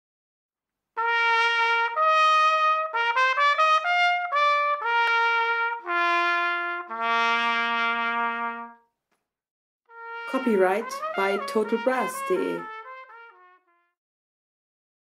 Tuyama® TTR 171 Trompete - Klangbeispiel
A short soundsample of our TTR-171 trumpet in Bb.

Kurze Präsentation des Sounds unserer TTR-171 Trompete in B. Mehr Infos finden Sie